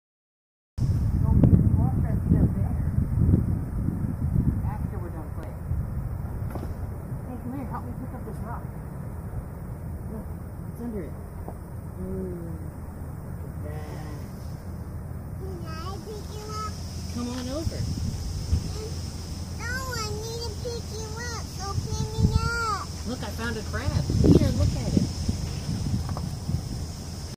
Unzen City, Nagasaki JAPAN : Ocean Ripples
The beauty of nature in our village in Unzen City, my family's hometown as of 2013 while I am with the JET Programme - (Shimabara Peninsula, Tachibana Bay, Nagasaki Prefecture, Japan)